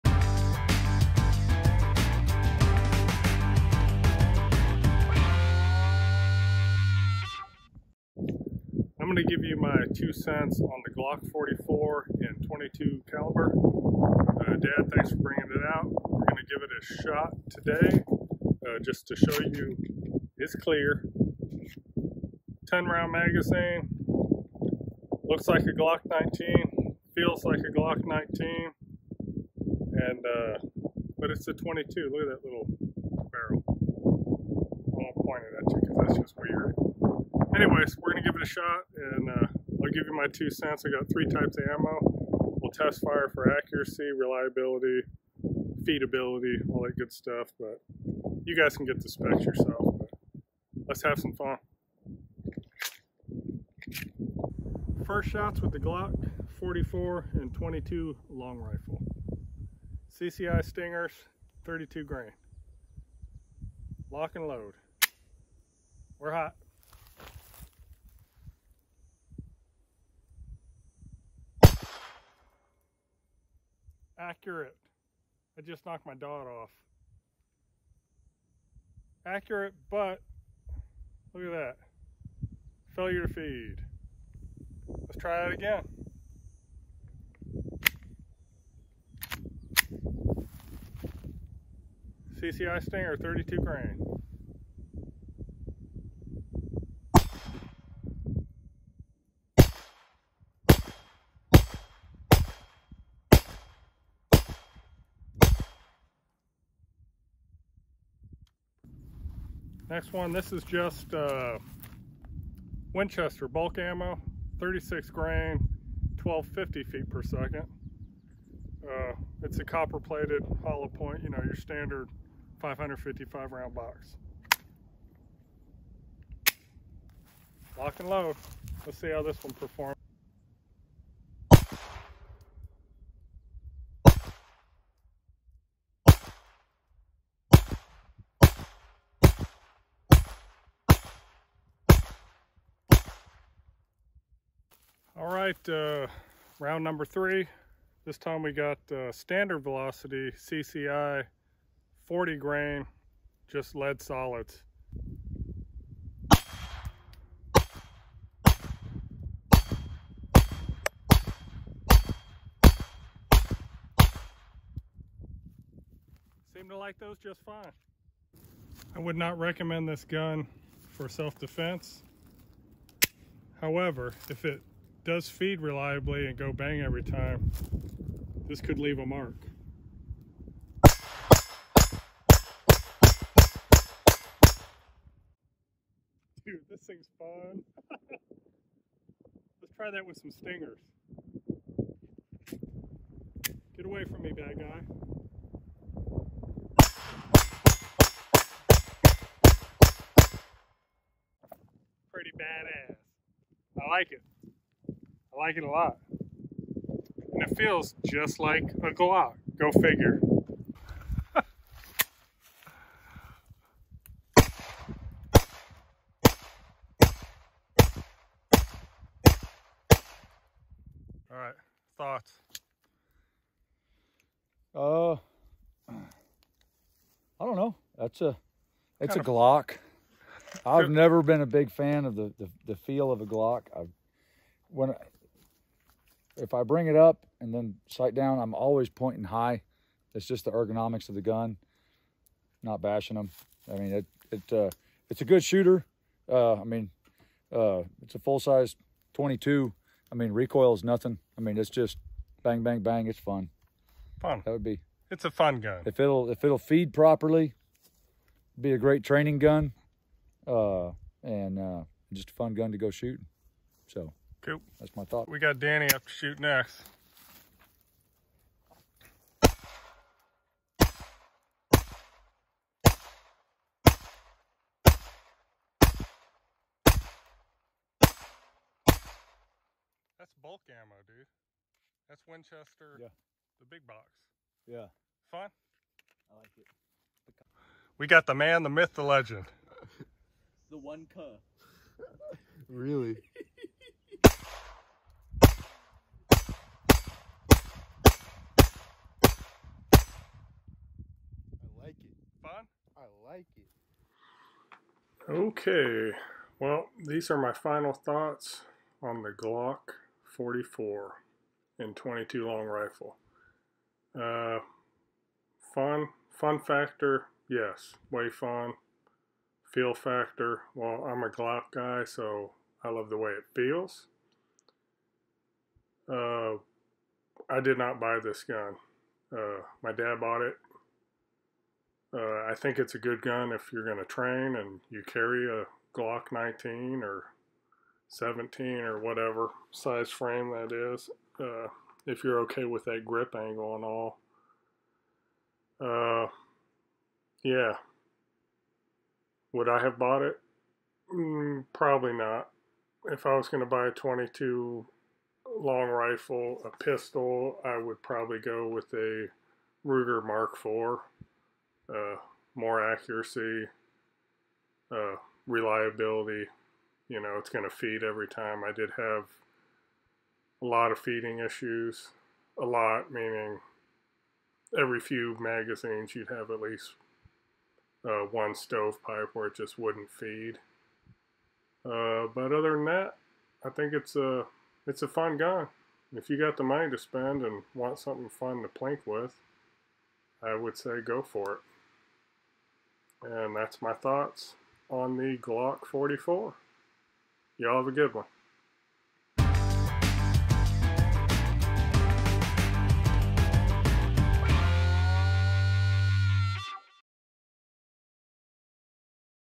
Glock 44 RANGE / REVIEW
First time shooting the Glock 44.  I shot it one day and had some friends shoot it a week later.  Sorry, the audio sucks cuz I wasn't planning on doing a video at the time.